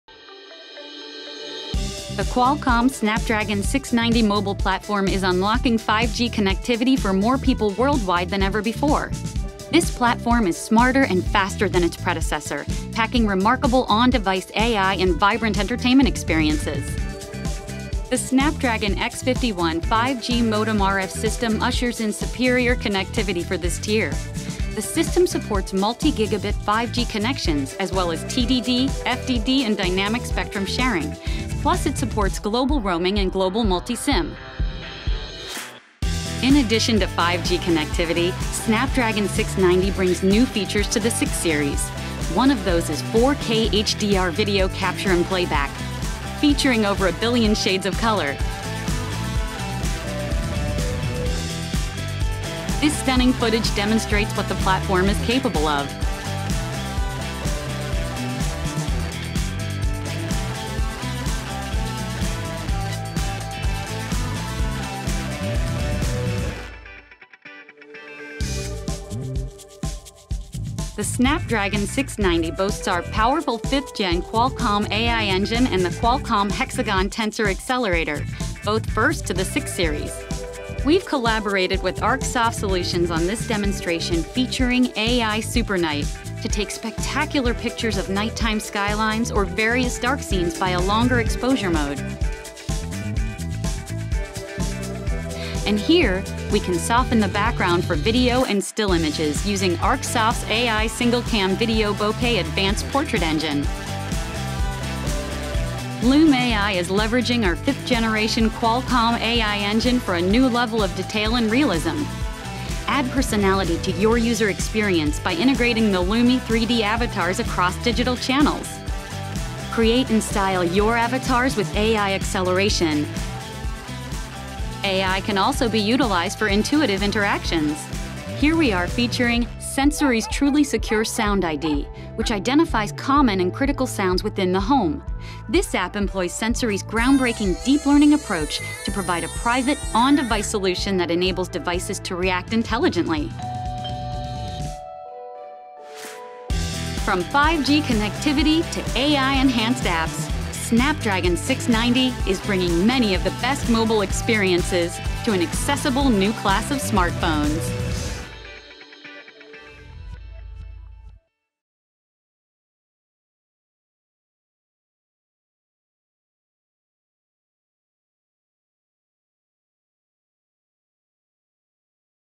Do it all with smartphones powered by the Snapdragon 690 5G Mobile Platform
From 5G connectivity and AI enhancements to next-gen camera features like 4K HDR video capture and playback, the Snapdragon 690 5G Mobile Platform is bringing many of the best mobile experiences to a new and accessible class of smartphones. Learn more about the platform: